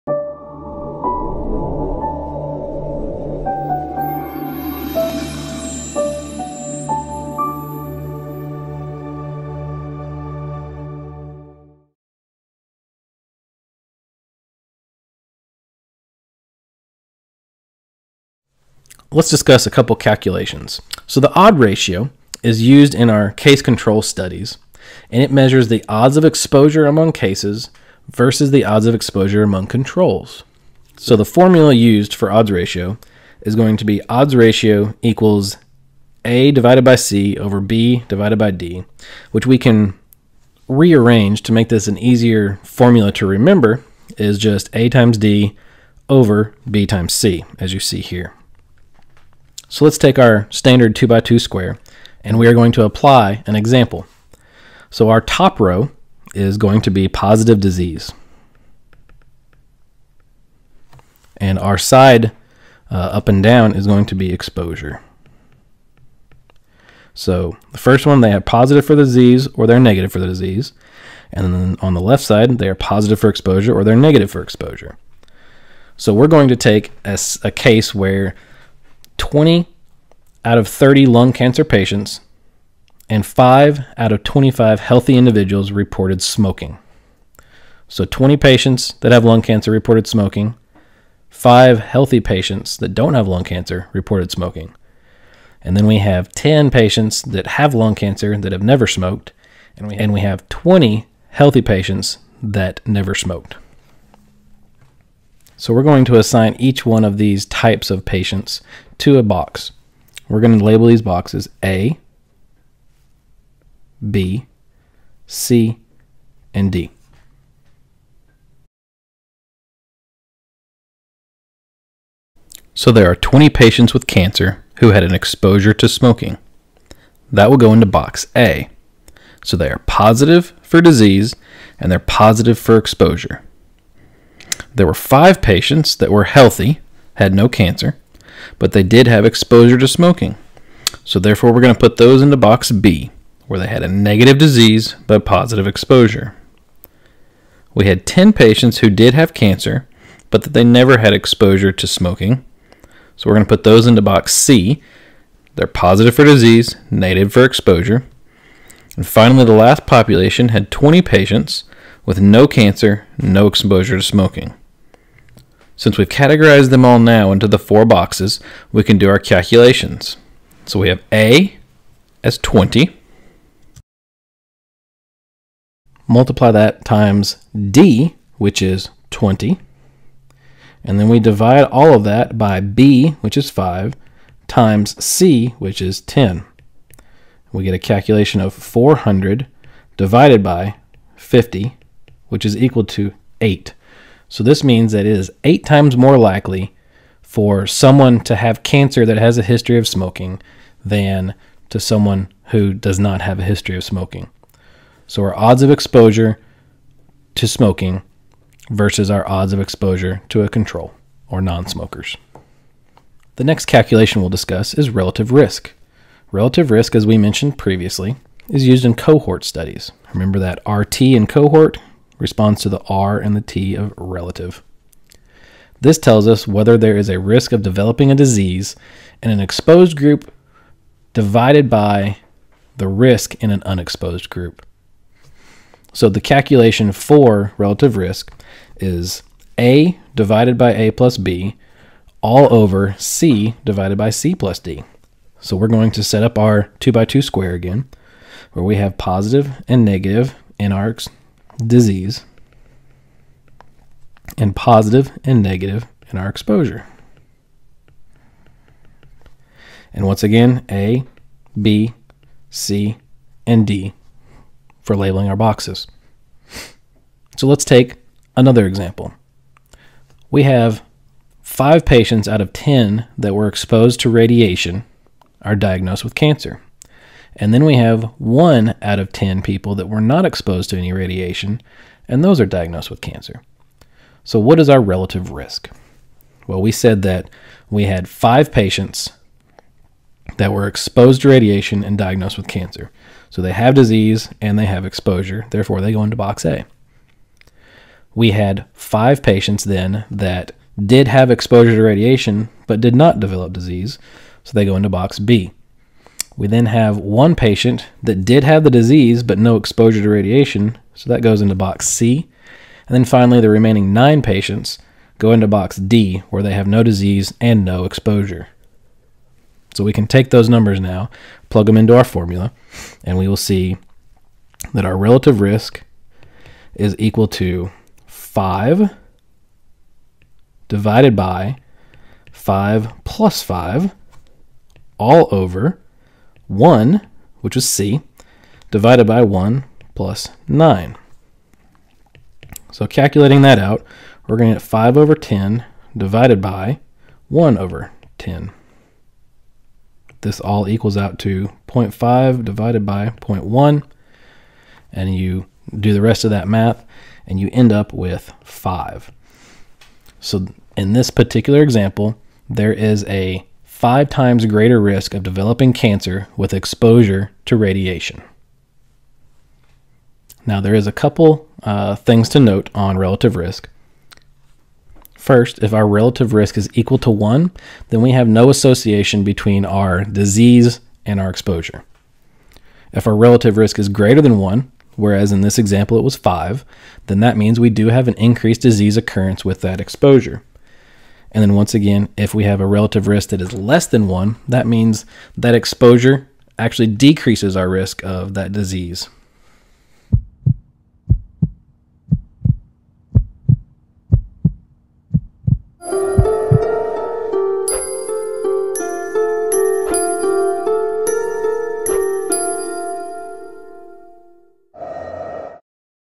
USMLE Step 1 | Behavioral Sciences | MedSmarter | S1: Part 3 of 5 | Odds Ration & Relative Risk
USMLE Step 1 Behavioral Sciences discussion from MedSmarter with Dr. Jason Colip.

The attached video is part 3 of 5, in a Behavioral Sciences series and discusses Odds Ratio & Relative Risk.  We break down the concepts in simple terms in shorter videos so students can focus on the Educational Objective to get a deep understanding of the concept.